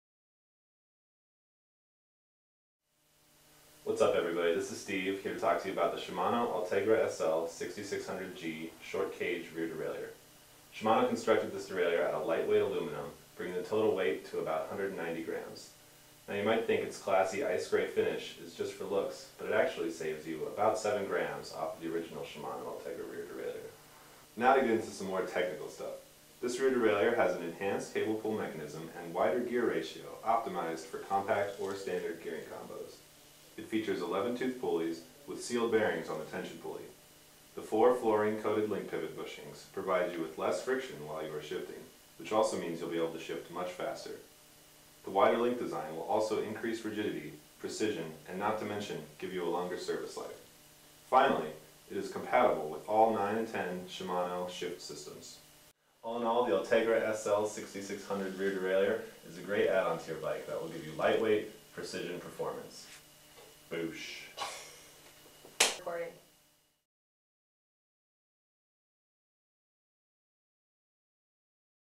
Shimano Ultegra SL 6600G Rear Derailleur Short Cage
The Shimano Ultegra SL 10 speed rear derailleur is durable, reliable, and comes in a stylish ice-grey finish.